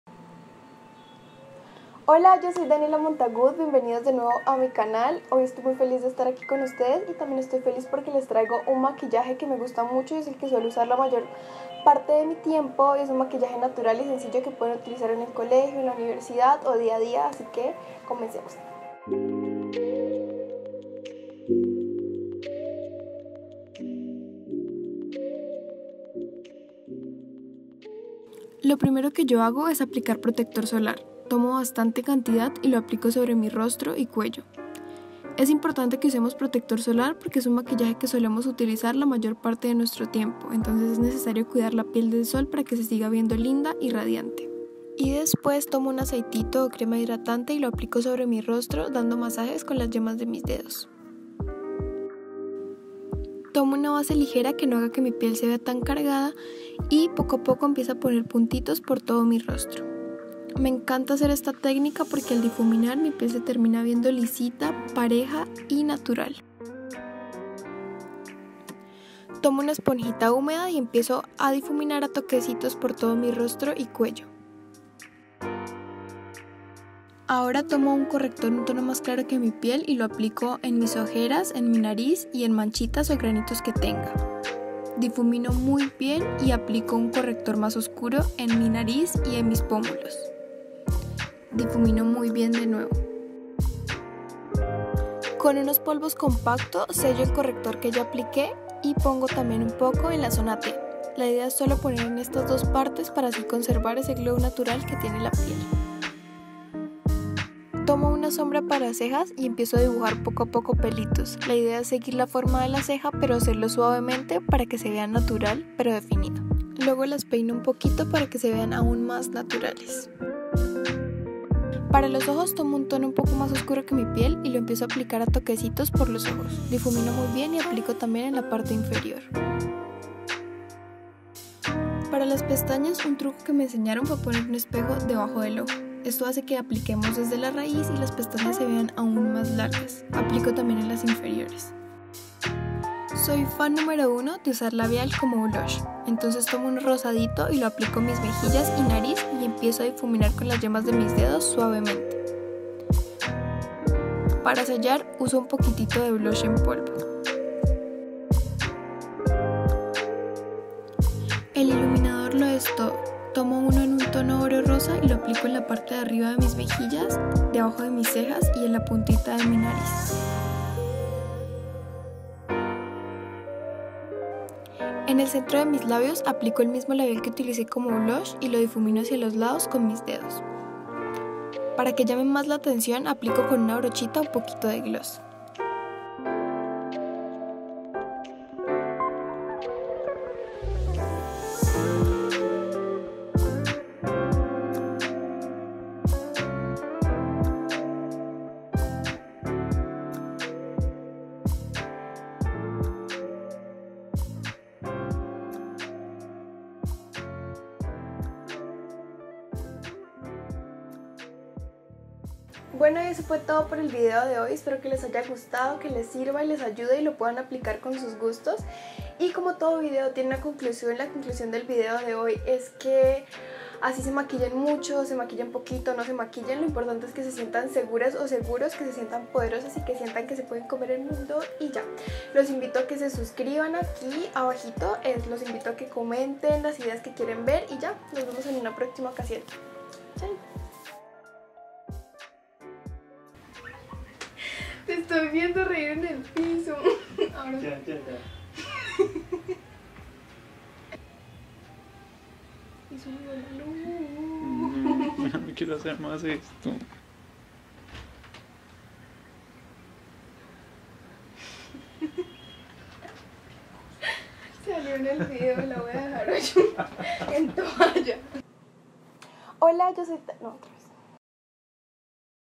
MAKEUP NO-MAKEUP // Maquillaje súper natural para principiantes
VOLVIIIIII 💖

Estoy muy feliz de estar de nuevo con ustedes.

Este es un maquillaje muy muuuuy muy natural que pueden usar para ir a la escuela, colegio, universidad y a todos los lugares posibles 💖🌛. Ya saben que la idea de este maquillaje no es ocultar que nos maquillamos, sino hacer un maquillaje fácil, rápido y lindo con muy pocos productos.
✨No olviden que si se hacen este maquillaje, uno cargado, uno colorido, o no se hacen nada, es por ustedes, para sentirse mágicas, hermosas, poderosas e increíbles.✨

Seré yo misma o no seré.